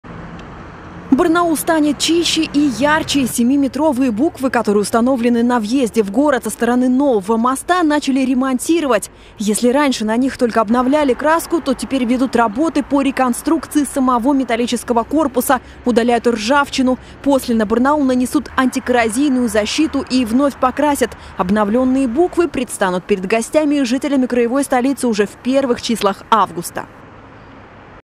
Буквы «Барнаул» на новом мосту начали ремонтировать
Барнаул станет чище и ярче. Семиметровые буквы, которые установлены на въезде в город со стороны нового моста начали ремонтировать. Если раньше на них только обновляли краску, то теперь ведут работы по реконструкции металлического корпуса, удаляют ржавчину. После на «Барнаул» нанесут антикоррозийную защиту и вновь покрасят. Обновлённые буквы предстанут перед гостями и жителями краевой столицы уже в первых числах августа.